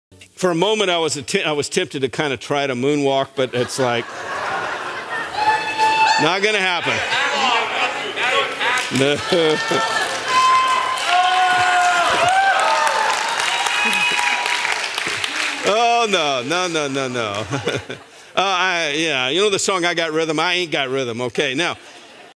Barney's Got Rythm
SRCC Pastor Barney Cargile shows us his moonwalking skills!